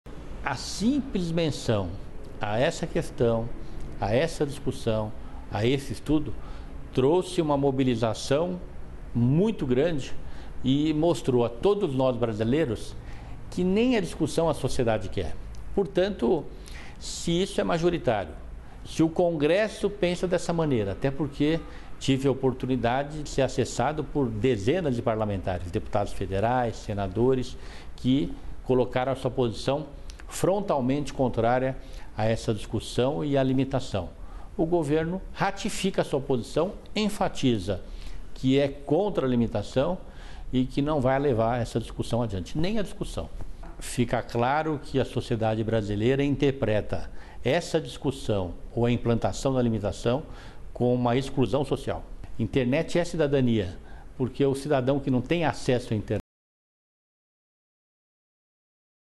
CGI.br endossa Kassab, para quem franquia na internet é exclusão social
Sob protestos da Anatel, o Comitê Gestor da Internet aprovou uma nota de apoio às declarações do ministro Gilberto Kassab, de Ciência, Tecnologia, Inovações e Comunicações, sobre os limites de franquia nas conexões à internet. Não as primeiras, naturalmente, quando o ministro disse que ia ter franquia e pronto, mas a retratação. Para Kassab, a reação foi tamanha que o governo entende a questão superada: nada de franquia.

“A simples menção a essa questão, a essa discussão, a esse estudo trouxe uma mobilização muito grande e mostrou a todos nós brasileiros que nem a discussão a sociedade quer”, afirmou o ministro em entrevista para a TV Brasil, ao revelar que foi muito pressionado depois que sugeriu o contrário, que a franquia seria adotada como regra.

“Se isso é majoritário, se o Congresso pensa dessa maneira, até porque fui acessado por dezenas de parlamentares, deputados federais e senadores, que colocaram sua posição frontalmente contraria a essa discussão e a limitação. O Governo ratifica sua posição, enfatiza que é contra a limitação e que não vai levar essa discussão adiante; nem a discussão. Fica claro que a sociedade brasileira interpreta essa discussão ou a implantação da limitação como exclusão social. Internet é cidadania, porque o cidadão que não tem acesso à Internet, ele está em condição de desigualdade com os outros cidadãos.”

Como que para sublinhar o posicionamento, o CGI.br aprovou em sua mais recente reunião, na sexta, 10/2, uma nota na qual declara “acompanhar a manifestação do Exmo. Senhor Ministro que, sensível às manifestações recebidas, pronunciou-se publicamente” e ainda sobre “como as recentes discussões a respeito da aplicação de franquia de dados nos acessos à Internet em banda larga fixa impactam na exclusão social”.

A Anatel, que ocupa um dos 21 assentos do CGI.br, reclamou. É que a agência tem um estudo em aberto sobre o tema da franquia, tendo em vigor uma cautelar do Conselho Diretor da agência que impede as operadoras de adotarem reduções de velocidade ou bloqueio das conexões mesmo em caso de consumo das franquias atualmente previstas em contratos. É certo que a posição da Anatel tem sido de tratar o assunto sem nenhuma pressa. Mas o muxoxo acabou sendo endereçado na mesma nota do Comitê Gestor, que conclui:

“O CGI.br se alinha à manifestação do Exmo. Sr. Ministro e considera oportuno que nenhuma decisão sobre o tema seja tomada sem que haja um amplo estudo técnico, jurídico e econômico com validade legal, teórica e empírica, observando-se também a experiência internacional a respeito, informando também que continuará a promover estudos, pesquisas e recomendações que visem à segurança dos usuários no uso da Internet e o permanente estimulo a sua crescente e adequada utilização pela sociedade.” Assistam a declaração do ministro da Ciência, Tecnologia, Comunicações e Inovações, Gilberto kassab.